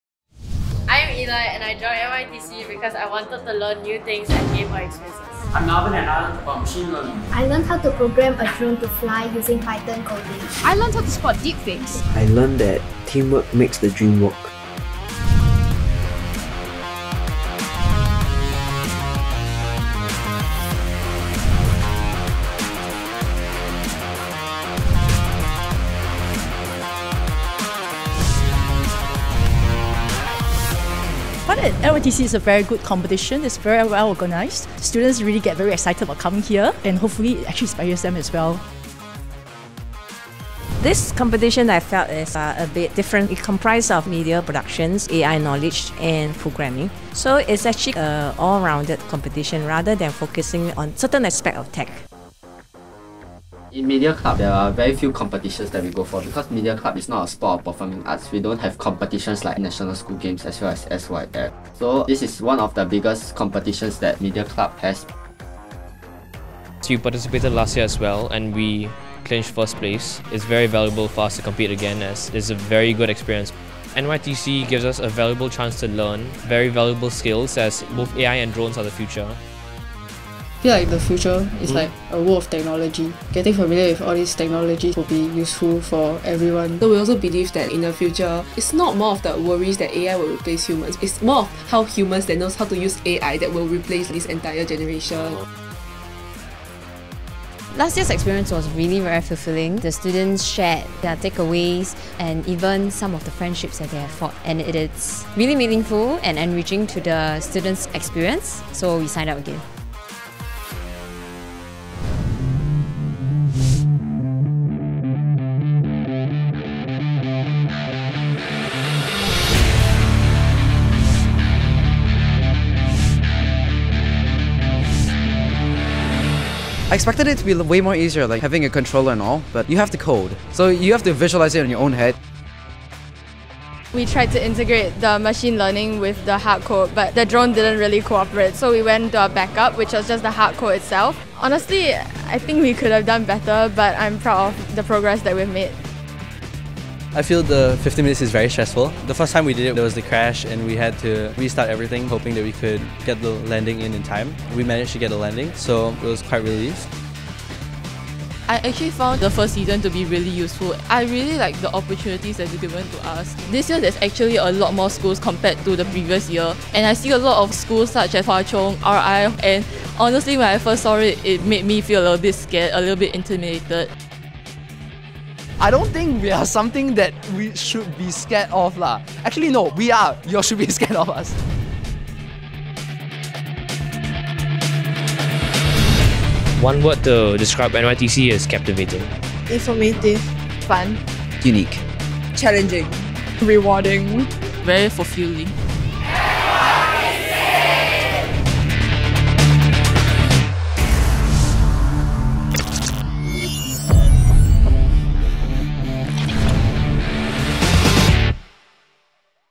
National Youth Tech Championship 2024 Highlights
Exclusive highlights on the National Youth Tech Championship 2024 Season 2: AI Drone Derby.